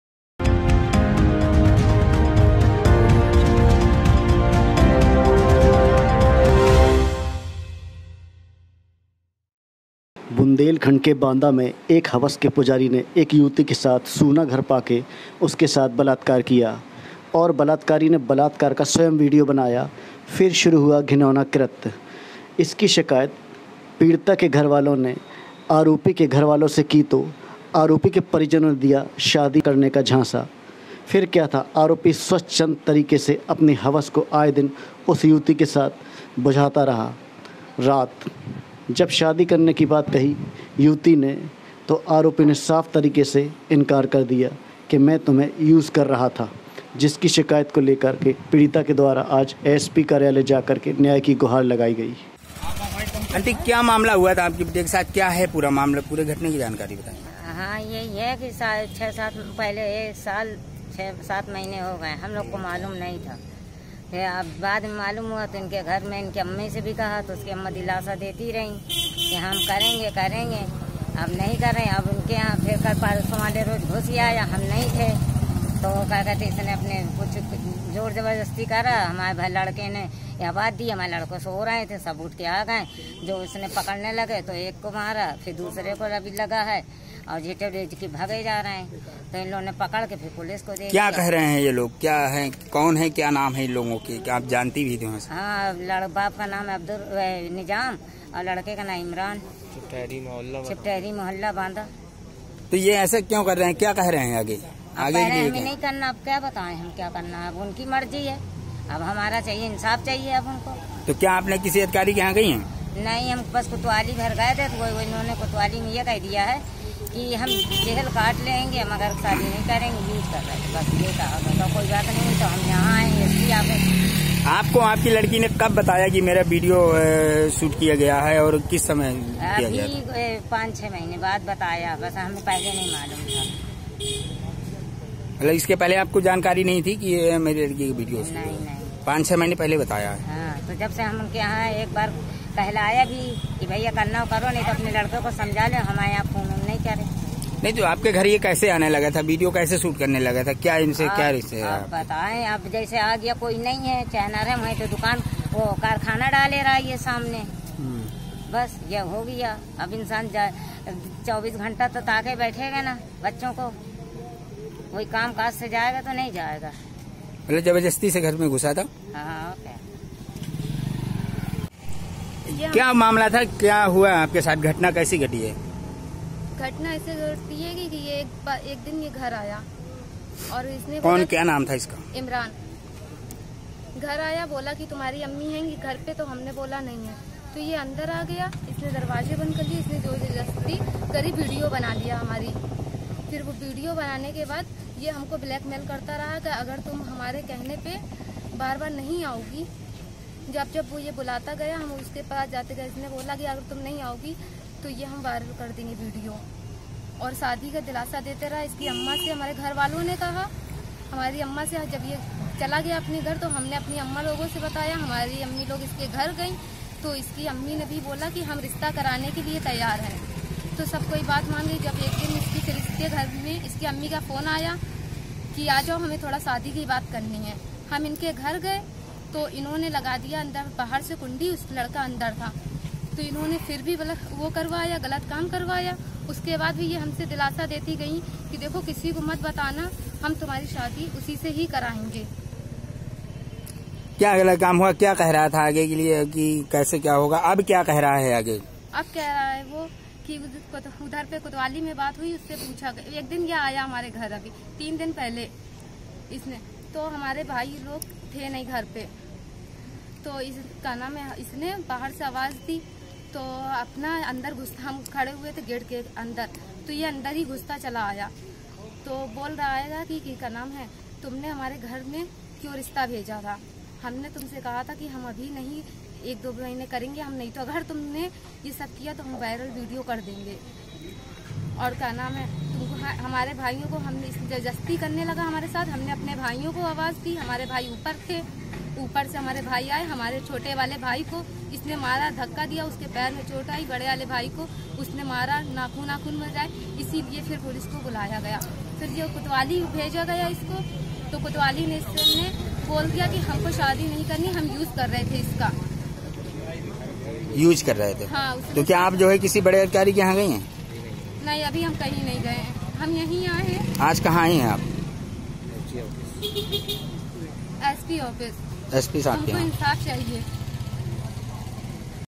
बाँदा : बलात्कारी ने बलात्कार का बनाया वीडियो वायरल करने की दे रहा है।
बलात्कारी ने बलात्कार का बनाया वीडियो वायरल करने की दे रहा है धमकी।

शिकायत पर घर वालों ने दिया शादी का आश्वासन, फिर लगातार बना रहा था यौन संबंध।

हवस के पुजारी ने अब शादी से किया इंकार पीड़िता पहुंची एसपी के पास।

बांदा में एक हवस के पुजारी ने एक युवती के साथ सोना घर पा करके उसके साथ बलात्कार किया और बलात्कारी ने बलात्कार का स्वयं वीडियो बनाया फिर शुरू हुआ घिनौना कृत्य घर वालों ने शिकायत घरवालों से की तो आरोपी के परिजनों ने दिया।

शादी करने का झांसा फिर क्या था आरोपी स्वच्छंद तरीके से अपनी हवस को आए दिन उस युवती के साथ बुझाता रहा रात में जब शादी करने की बात कही यूपी ने तो आरोपी ने साफ तरीके से इंकार कर दिया कि मैं तुम्हें यूज कर रहा था जिसकी शिकायत को लेकर कि पीड़िता के द्वारा आज पुलिस अधीक्षक कार्यालय जाकर के न्याय की गुहार लगाते हुए कार्यवाही की मांग की है।

बांदा नगर कोतवाली अंतर्गत का मामला है जहां की रहने वाली एक पीड़िता घर में अकेली थी और वहीं पास का रहने वाला इमरान नाम का एक युवक सूना घर पाकर पिडिता के घर पहुंचा और अपनी मम्मी के आने की सूचना पूछी साथ में घर में किसी को न पा कर युवती के साथ बलात्कार किया और इतना ही नहीं उस बलात्कार का वीडियो भी बना डाला साथ में युवती को धमकी भी दीया की अगर किसी से बताया तो इस वीडियो को वायरल कर दूंगा जिसकी शिकायत पीड़िता ने आज पुलिस अधीक्षक से की है

रिपोर्टर-इल्यास खान
बाँदा

Plz share with love